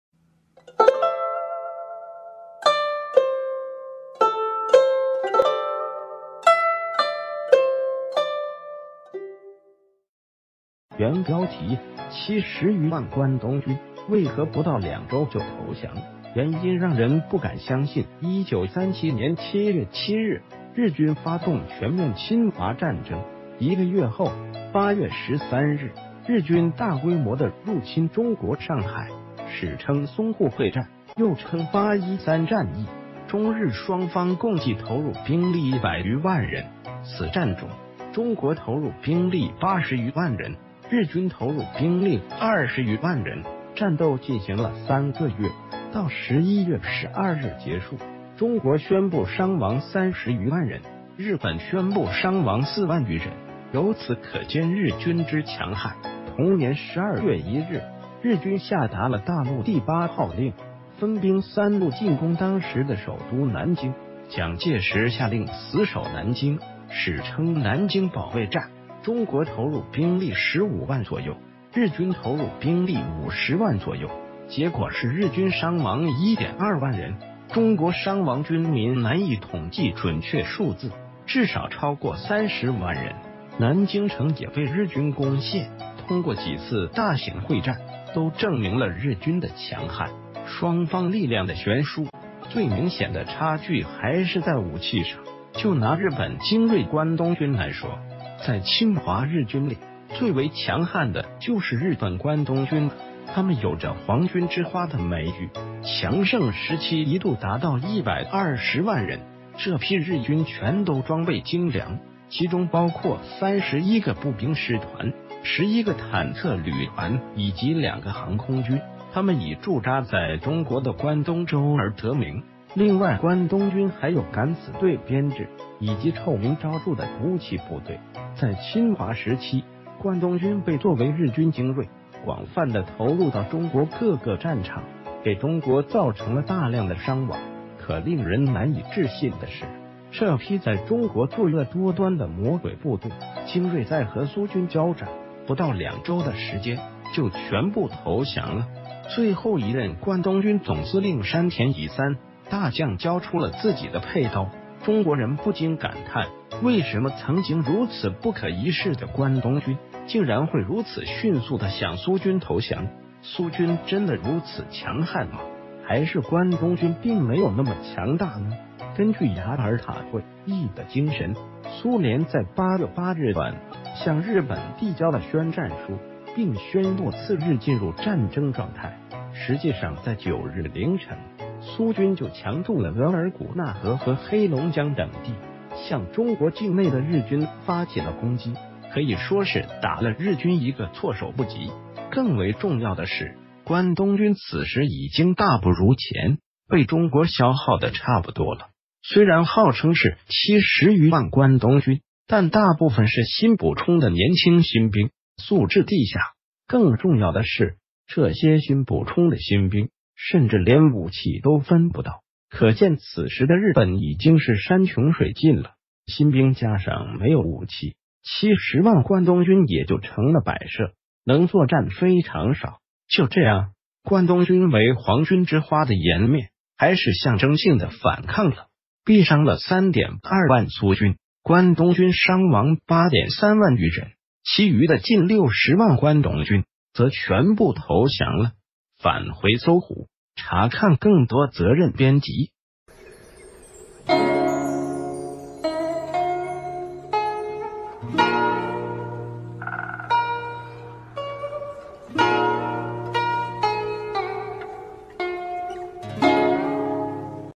70余萬關東軍，為何不到兩周就投降？原因讓人不敢相信_搜狐歷史_搜狐網
原標題：70余萬關東...